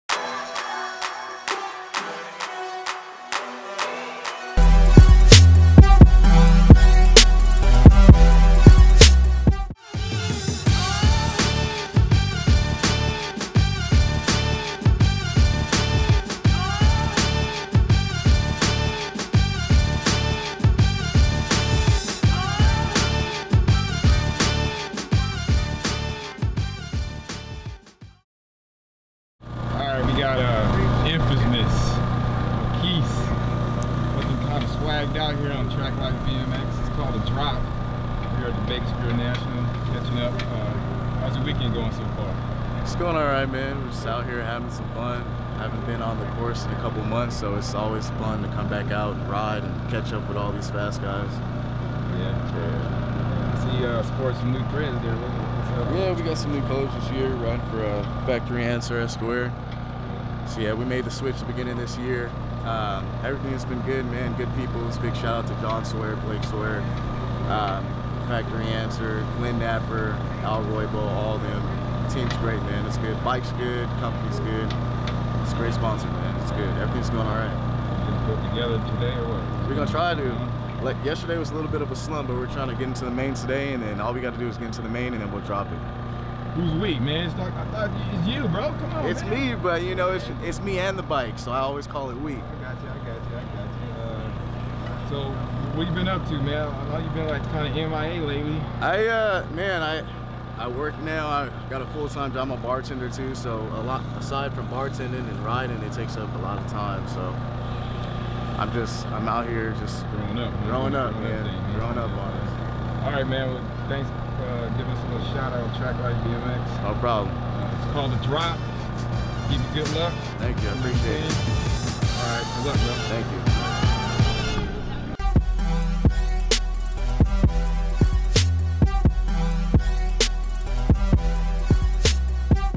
TRACKLIFEBMX: WHAT'Z COOKING? | MARQUISE MONTGOMERY
TracklifeBmx pinned up Factory Answer BMX  USABMX  A pro-Marquise Montgomery about what he's been up to. And why he was MIA.